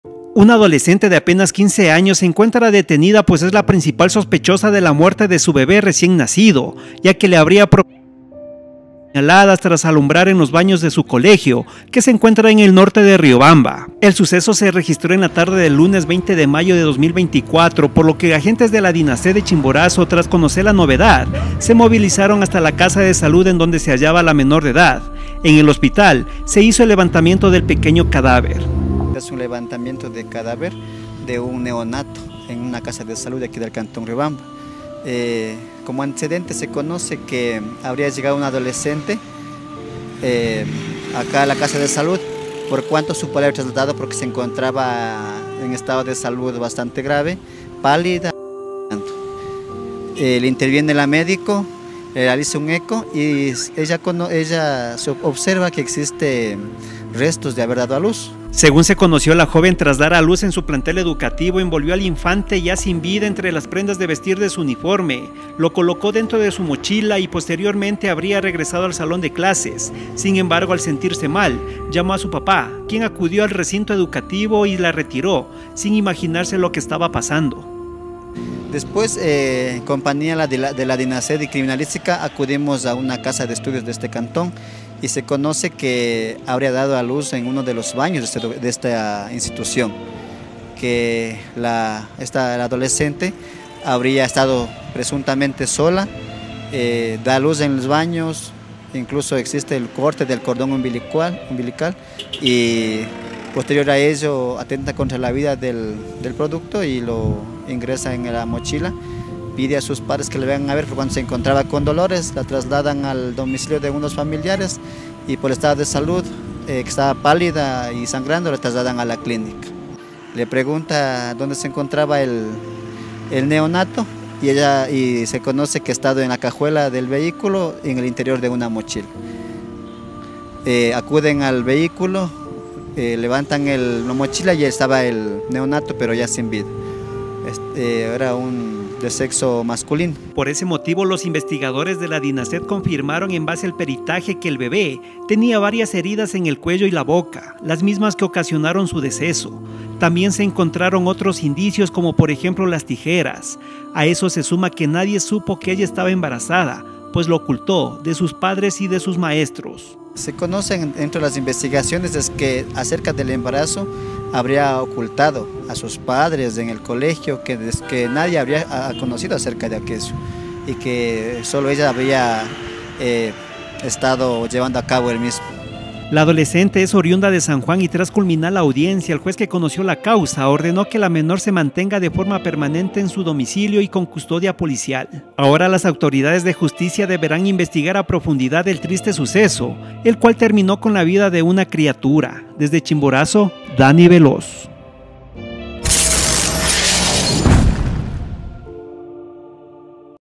Una adolescente dio a luz a un bebé en el baño de su colegio en Riobamba
Una adolescente de apenas 15 años, se encuentra con arresto domiciliario, pues es la principal sospechosa de la muerte de su bebé recién nacido, ya que le habría propinado al menos cuatro puñaladas tras alumbrar en los baños de un colegio que se encuentra en el norte de Riobamba.
Aquí la noticia.